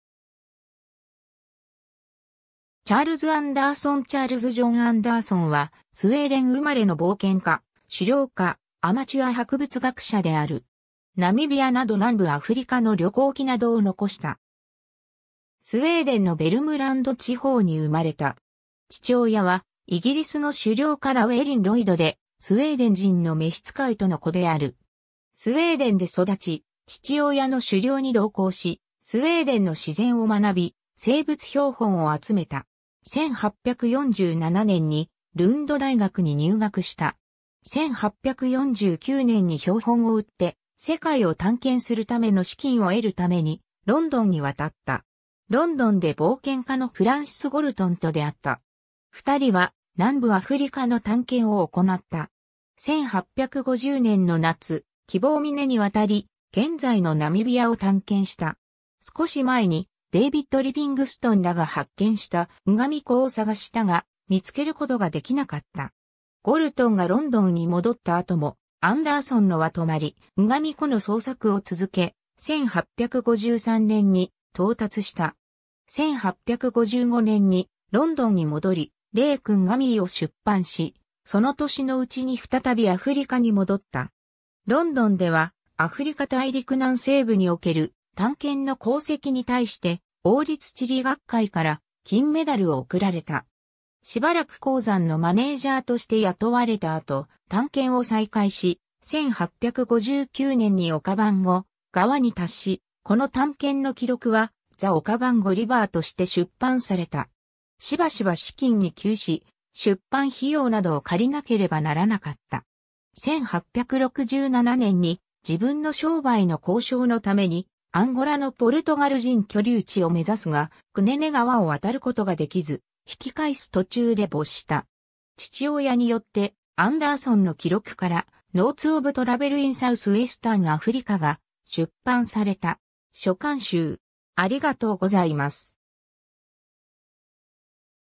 チャールズ・ジョン・アンダーソン
金メダル_(王立地理学会)_受賞者
Charles Andersson チャールズ・ジョン・アンダーソン（Charles John Andersson、またはKarl John (Karl Johan) Andersson、1827年3月4日 - 1867年7月9日）は、スウェーデン生まれの冒険家、狩猟家、アマチュア博物学者である。
ナミビアなど南部アフリカの旅行記などを残した。
 スウェーデンのヴェルムランド地方にうまれた。
父親はイギリスの狩猟家Llewellyn Lloyd で、スウェーデン人の召使いとの子である。
スウェーデンで育ち、父親の狩猟に同行し、スウェーデンの自然を学び、生物標本を集めた。
1847年にルンド大学に入学した。
1849年に標本を売って世界を探検するための資金を得るためにロンドンに渡った。
ロンドンで冒険家のフランシス・ゴルトンと出会った。
2人は南部アフリカの探検を行った。
1850年の夏、喜望峰に渡り、現在のナミビアを探検した。
少し前に、デイヴィッド・リヴィングストンらが発見したンガミ湖を探したが、見つけることができなかった。
ゴルトンがロンドンに戻った後も、アンダーソンのは留まり、ンガミ湖の捜索を続け、1853年に到達した。
1855年にロンドンに戻り、"Lake Ngami"を出版し、その年のうちに再びアフリカに戻った。
ロンドンでは、アフリカ大陸南西部における探検の功績に対して、王立地理学会から金メダルを贈られた。
 しばらく鉱山のマネージャーとして雇われた後、探検を再開し、1859年にオカヴァンゴ川に達し、この探検の記録は"The Okavango River"として出版された。
しばしば資金に窮し、出版費用などを借りなければならなかった。
 1867年に自分の商売の交渉のためにアンゴラのポルトガル人居留地をめざすが、クネネ川を渡ることができず引き返す途中で没した。
父親によって、アンダーソンの記録から"Notes of Travel in South-Western Africa"が出版された。
 書簡集